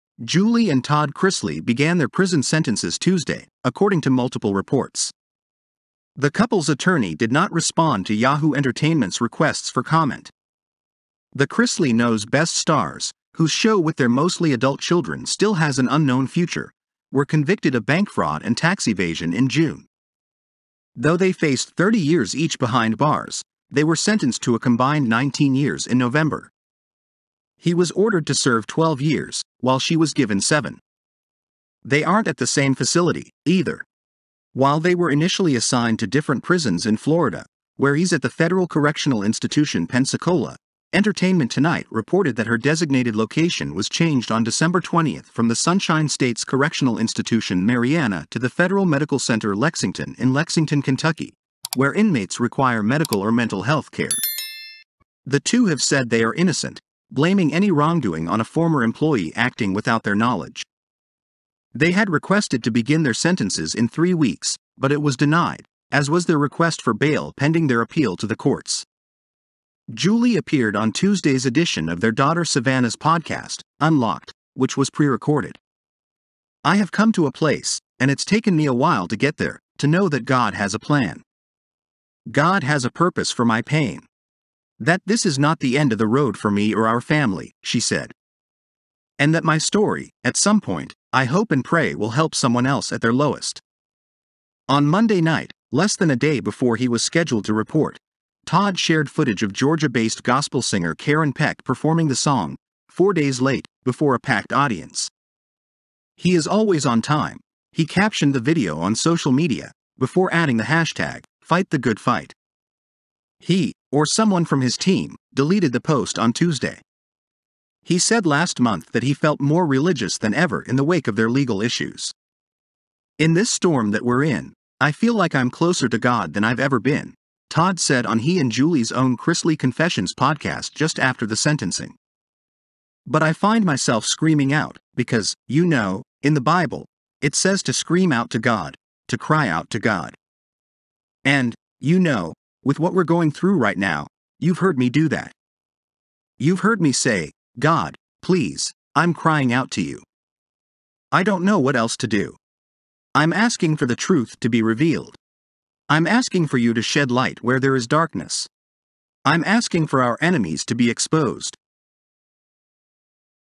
Todd and Julie Chrisley report to prison on bank fraud, tax evasion convictions
Julie and Todd Chrisley began their prison sentences Tuesday, according to multiple reports.